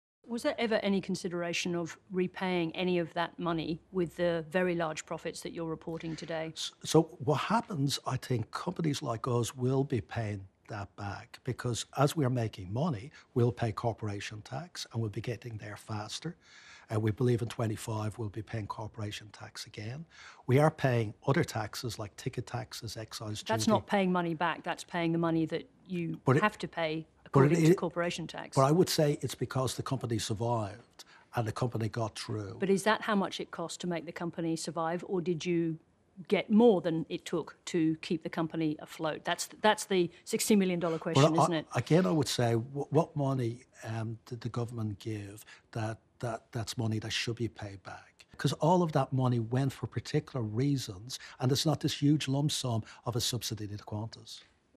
Should Qantas repay the taxpayer-funded COVID support they received?
On Thursday, Qantas unveiled a record pre-tax profit of $2.47 billion, partly fuelled by sky-high demand for domestic and international travel.

But despite that, CEO Alan Joyce told 7.30’s Sarah Ferguson there’s no reason for Qantas to repay the government funds it received during the height of the pandemic.